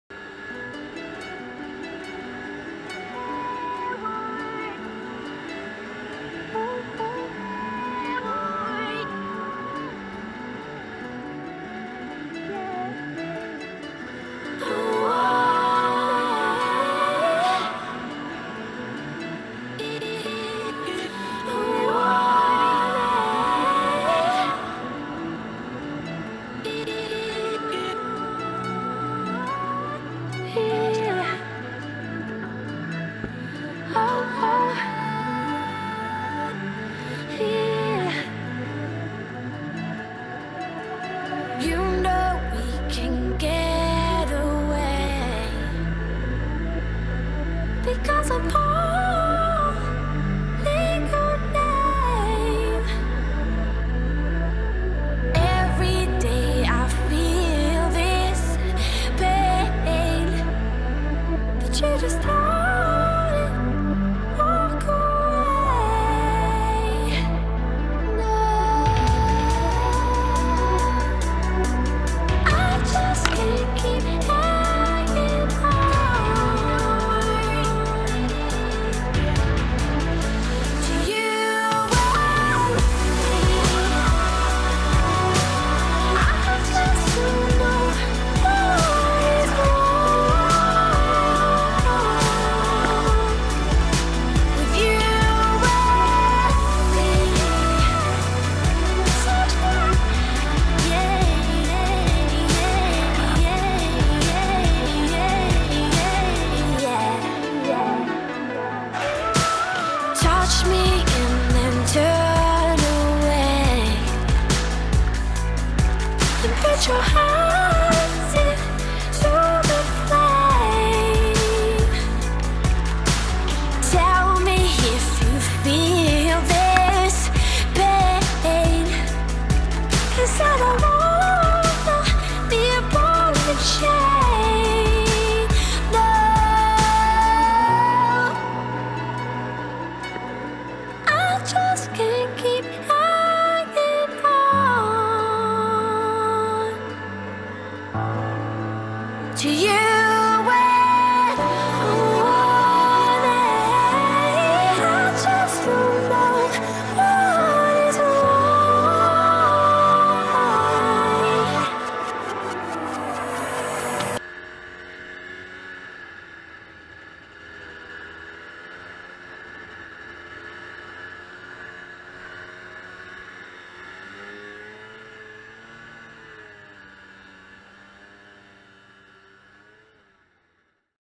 ski doo freestyle
camera test sjcam sj4000 wifi.  ski doo freestyle 300